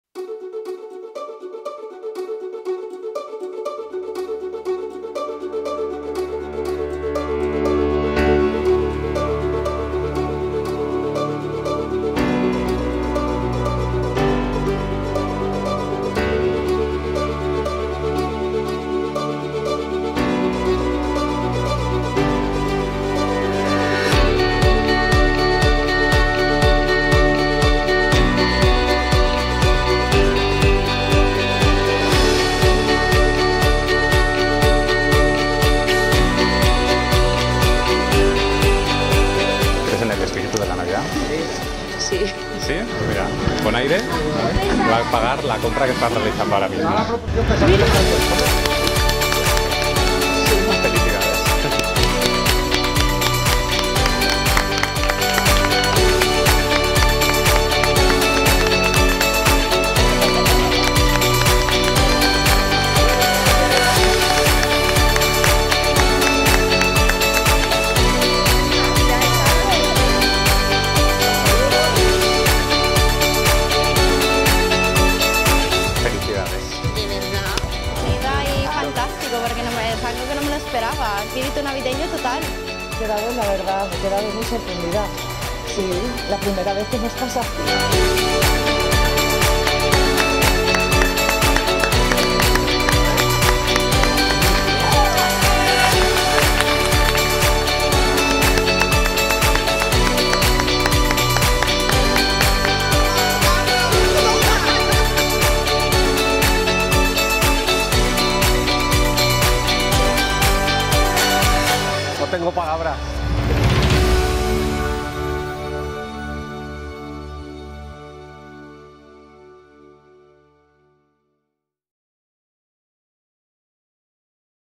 The Real Christmas Spirit | Bonaire Shopping Resort 🇪🇸
Can you imagine going shopping and having someone pay for your purchase? That is the magic of Christmas and that is what Unexpected Man is all about. This experience in the Bonaire Shopping Resort was a great success.